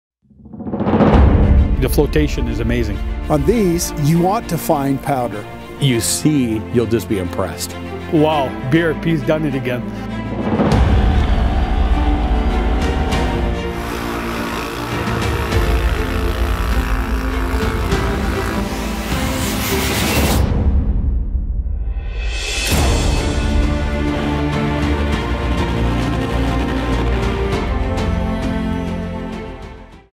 New Can-Am Apache Backcountry Track System for ATV - Short Video
Brand new track kit designed for deep backcountry snow and challenging winter conditions. Features: Largest contact patch, infused snowmobile technology, high ground clearance in deep snow and enhanced handling properties.